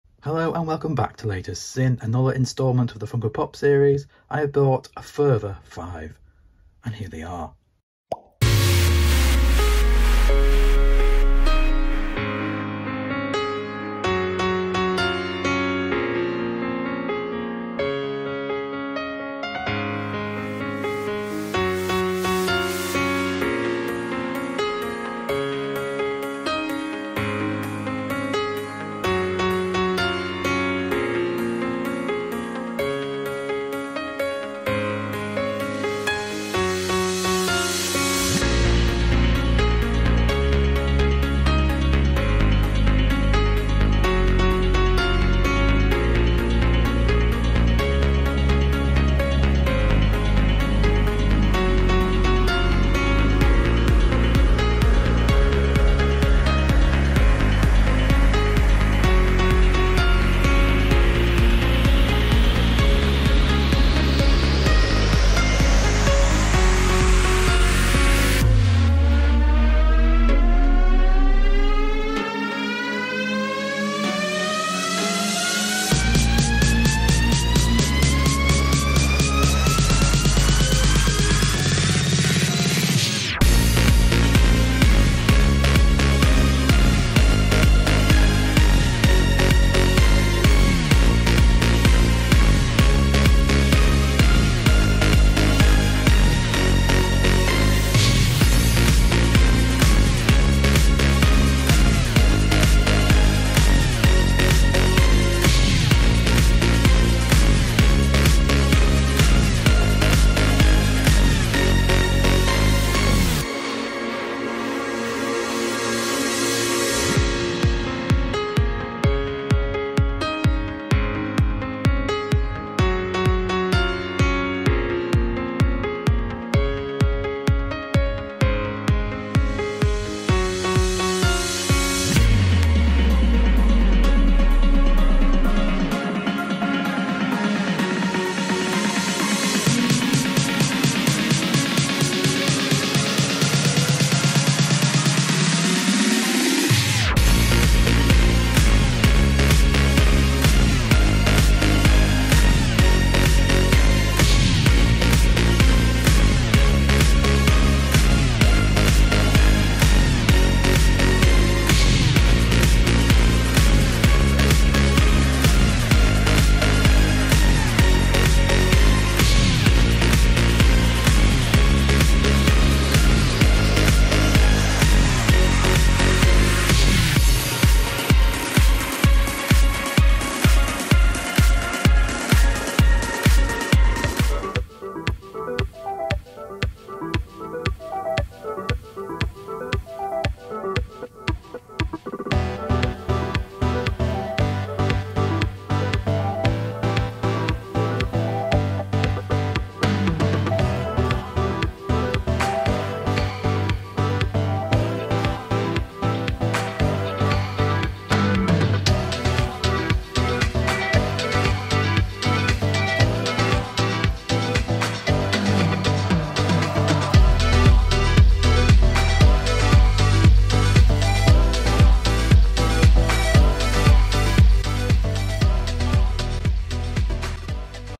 What?! Another 5 Funko Pops! | Pop Rocks | Marvel | DC
My Funko Pop collecting journey continues, another 5 Funko Pops added to the collection... Featuring: Pop Rocks, Marvel, DC - What did I buy?